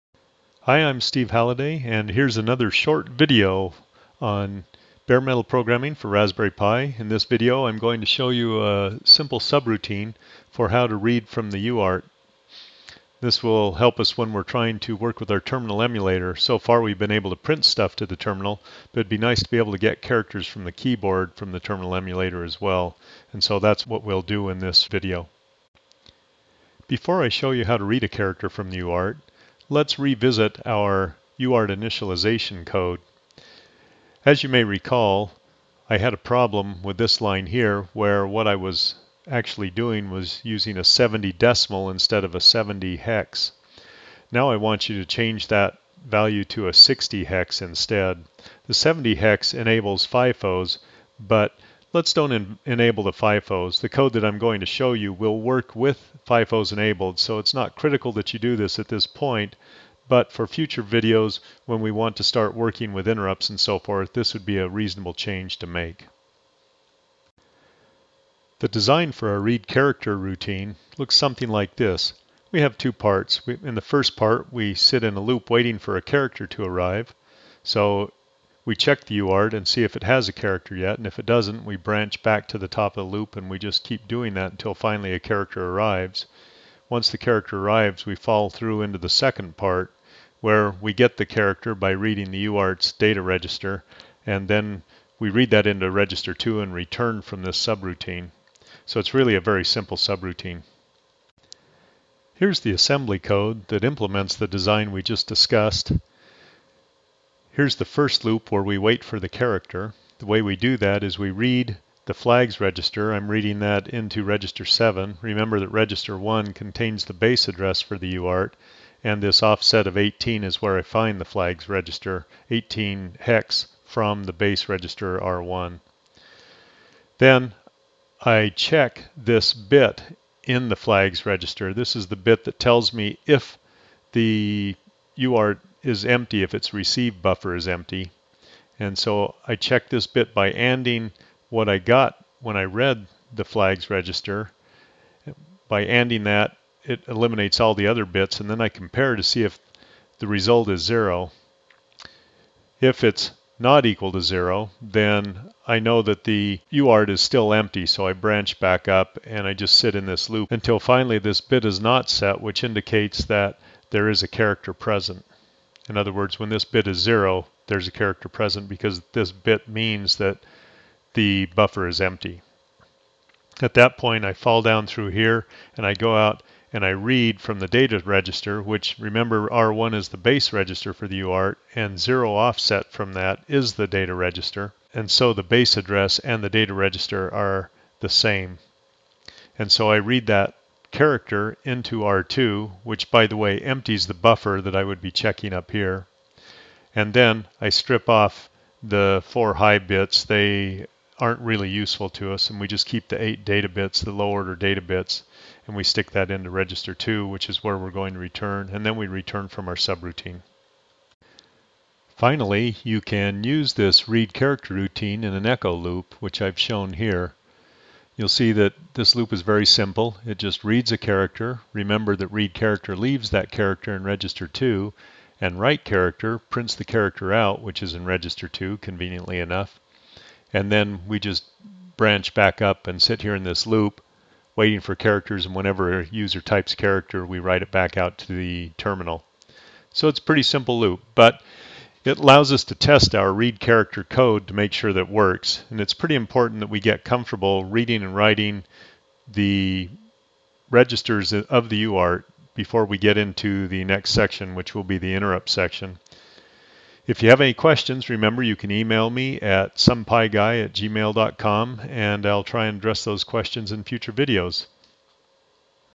Raspberry Pi - Reading from the UART
Raspberry Pi bare metal programming for reading from a universal asynchronous receiver transmitter (UART) -- Steve Halladay shows how to poll the UART and read characters.